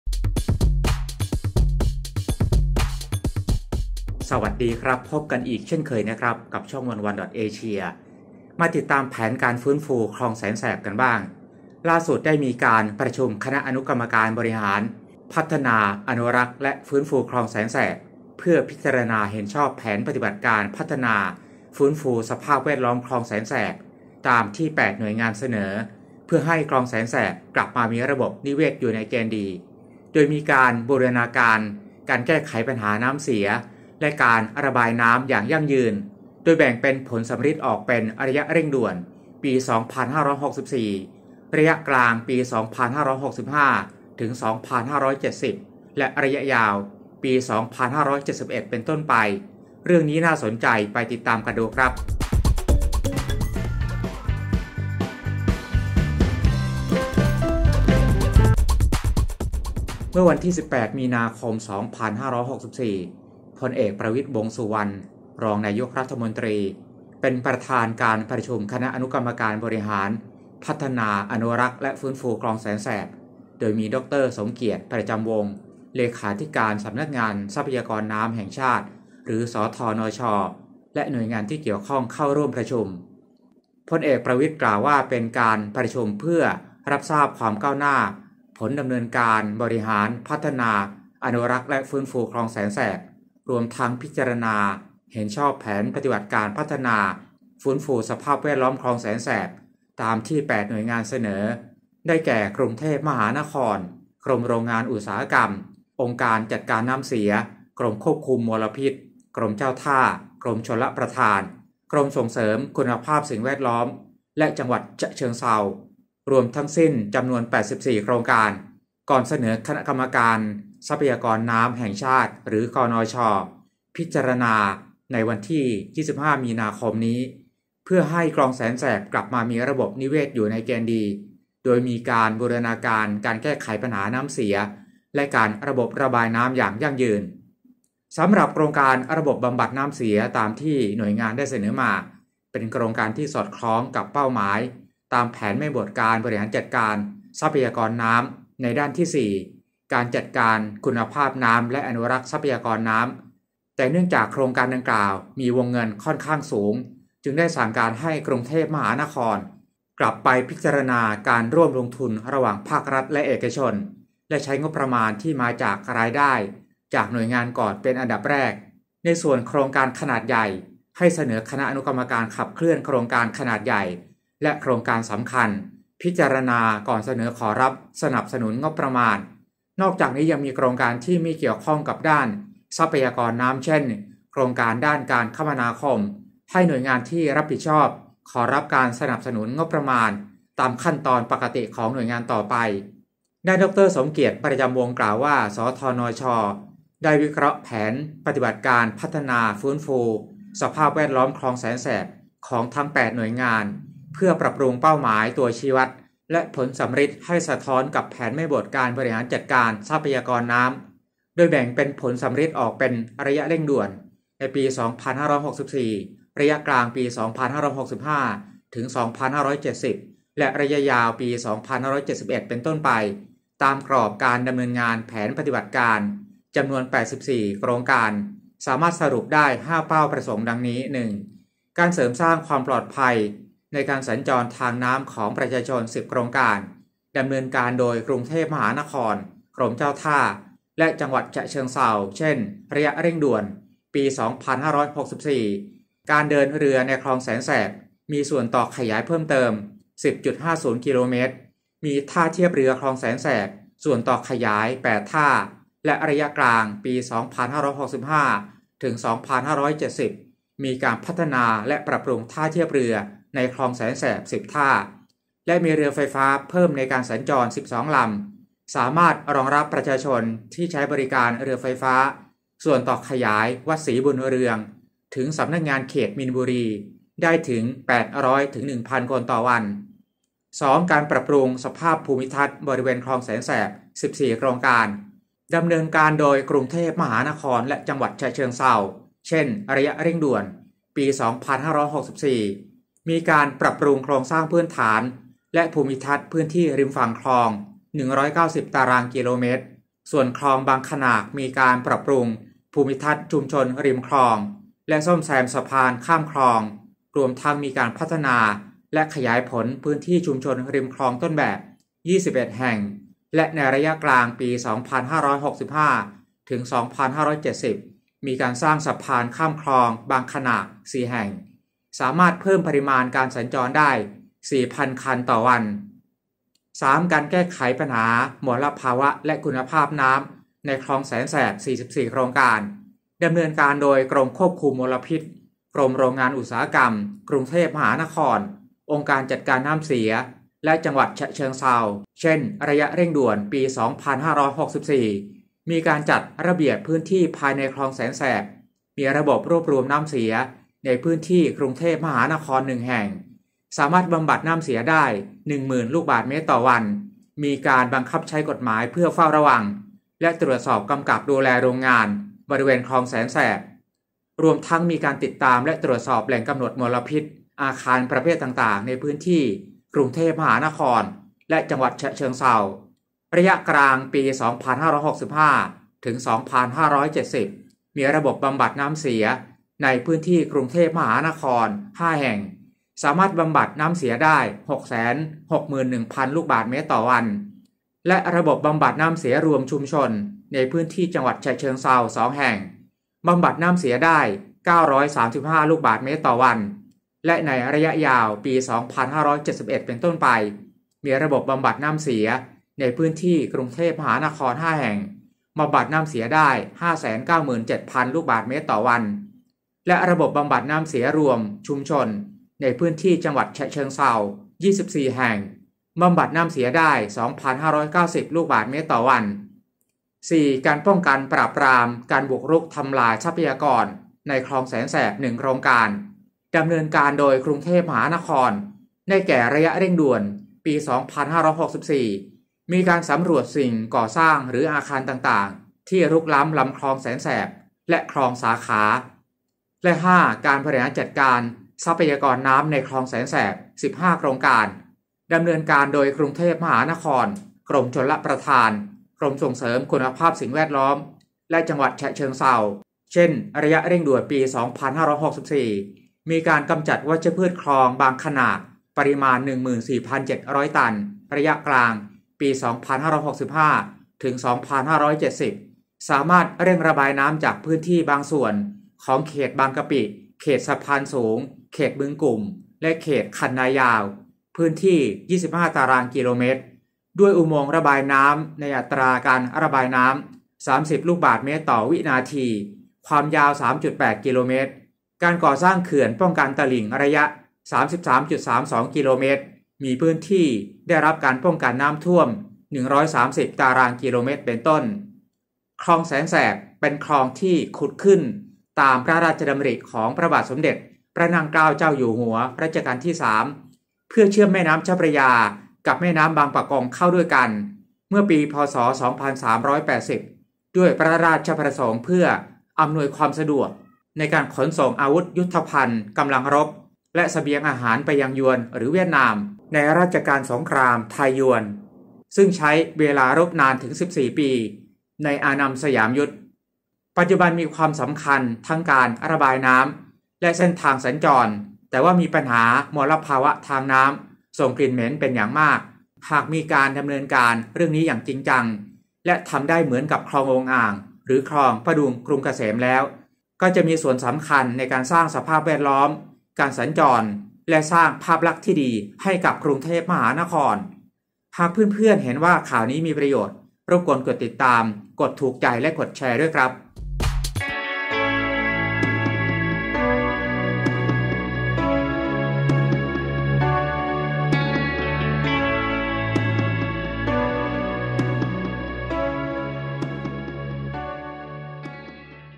ชงแผนปฏิบัติการพลิกโฉมคลองแสนแสบ ให้เห็นผลใน 3 ระยะ
สวัสดีครับพบกันอีกเช่นเคยกับช่อง 1one.asia มาติดตามการฟื้นฟูคลองแสนแสบ กันบ้าง ที่ล่าสุด ได้มีการประชุมคณะอนุกรรมการบริหาร พัฒนา อนุรักษ์ และฟื้นฟูคลองแสนแสบ เพื่อพิจารณาเห็นชอบแผนปฏิบัติการพัฒนา ฟื้นฟูสภาพแวดล้อมคลองแสนแสบ ตามที่ 8 หน่วยงานเสนอ เพื่อให้คลองแสนแสบกลับมามีระบบนิเวศอยู่ในเกณฑ์ดี โดยมีการบูรณาการ การแก้ไขปัญหาน้ำเสียและการระบายน้ำอย่างยั่งยืน โดยแบ่งเป็นผลสัมฤทธิ์ออกเป็นระยะเร่งด่วน (ปี 2564) ระยะกลาง (ปี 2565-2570) และระยะยาว (ปี 2571 เป็นต้นไป) เรื่องนี้น่าสนใจไปติดตามกันครับ คลิกอ่านรายละเอียด